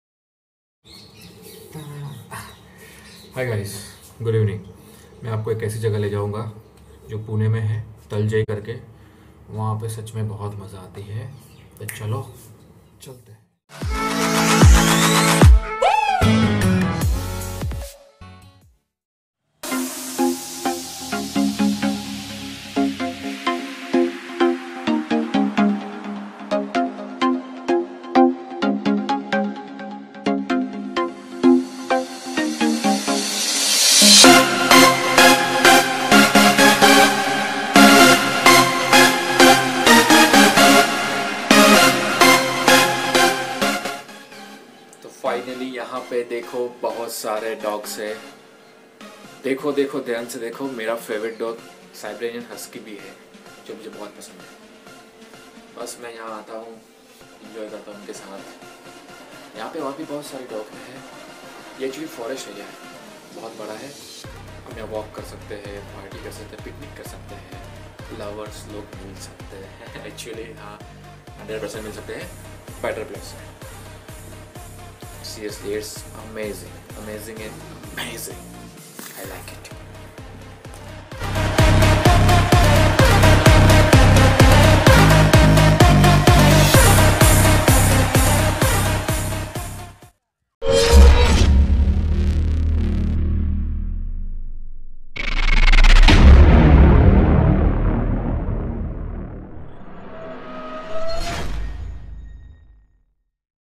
Pune ki ek jagah ... ( Taljai , forest park)
Hello guy's

Pune ki ek jagah taljai me waha Abhi hi kuch din pehle waha Gaya tha aur Mene shoot Kiya,
Sach me amazing jagah hai ..
Must visit guy's .